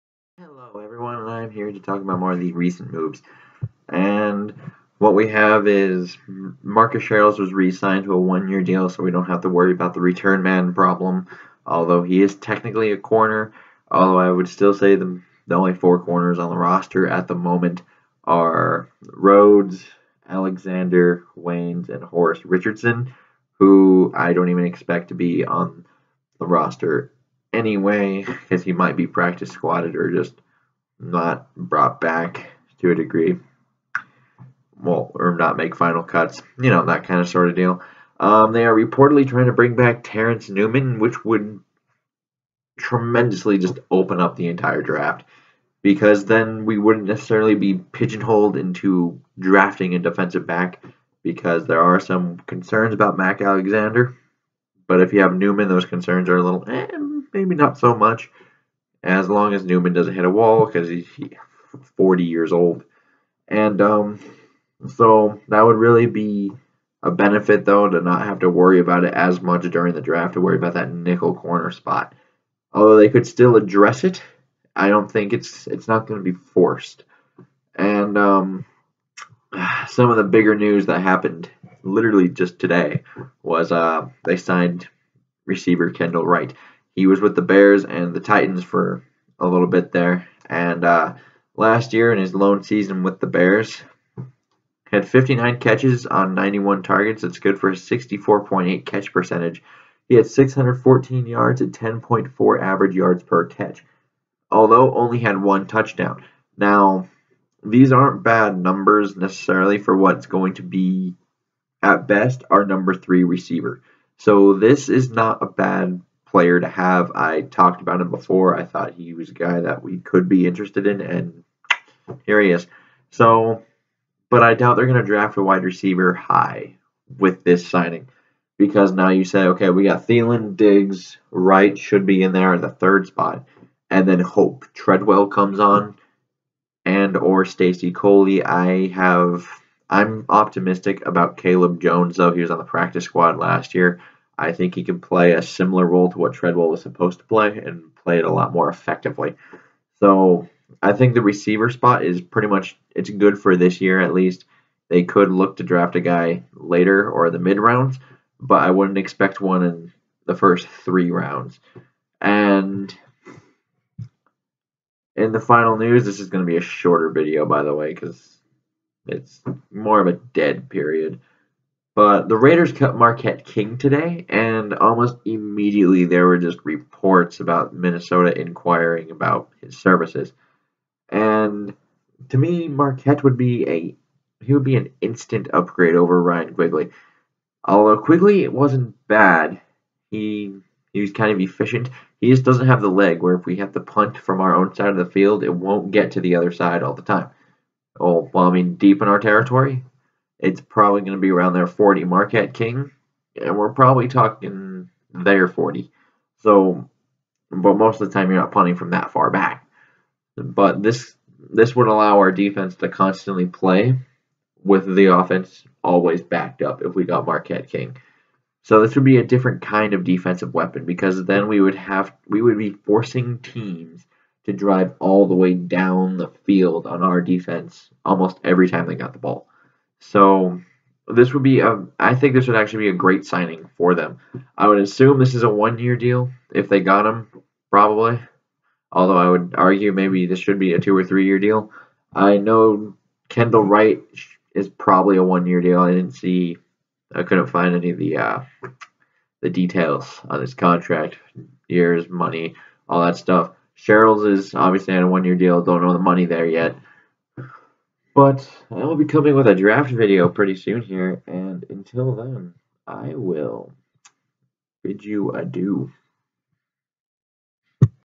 Signing of Kendall Wright, Sherels Coming Back, and the Recent Marquette King Speculation
I go over the recent happenings of the Minnesota Vikings and what things could mean by bringing in some of the guys they are bringing in.